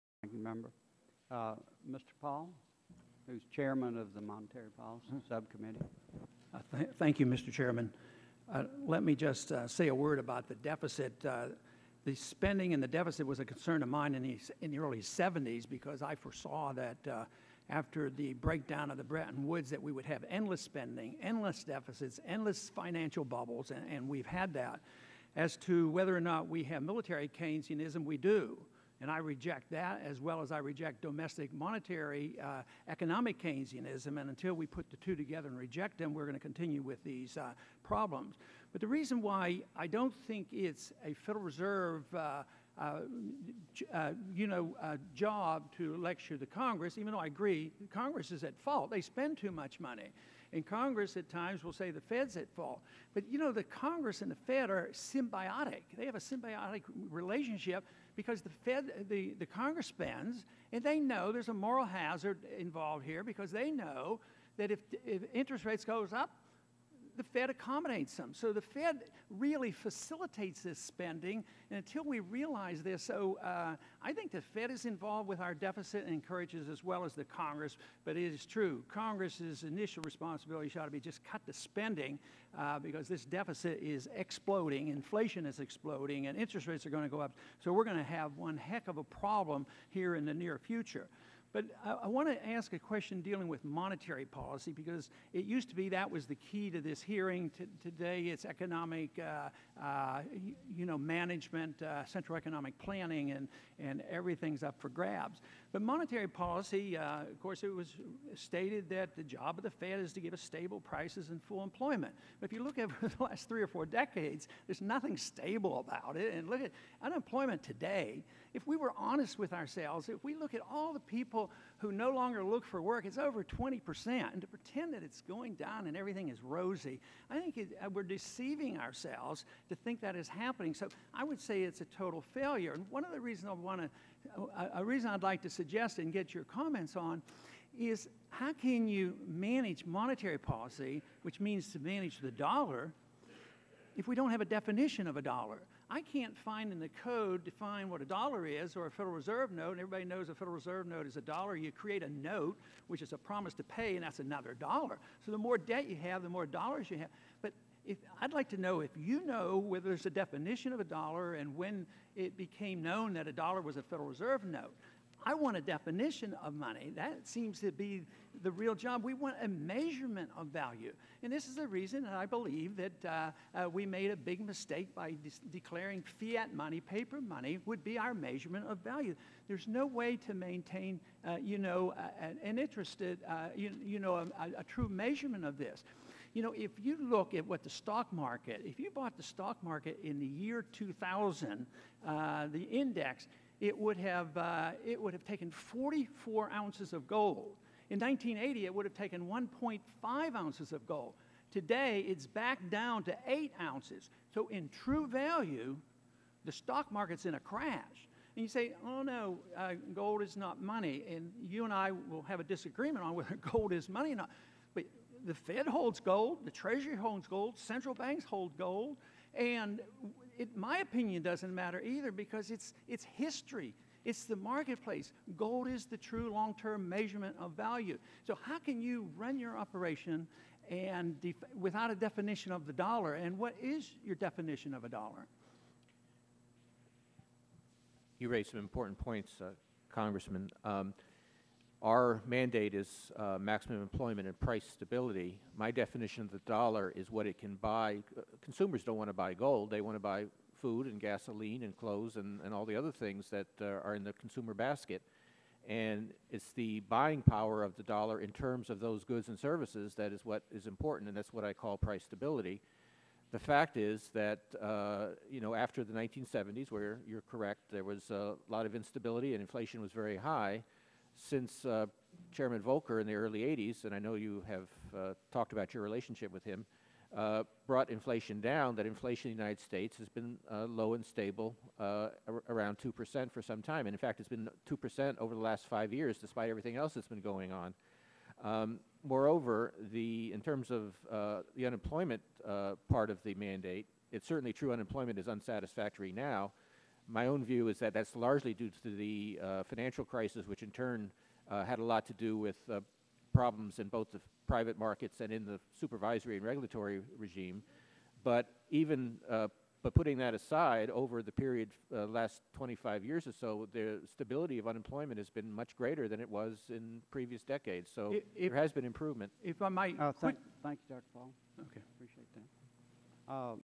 Rep. Paul Questions Chairman Bernanke on the Measurement of Money
Rep. Ron Paul questions Chairman Bernanke on the measurement of money.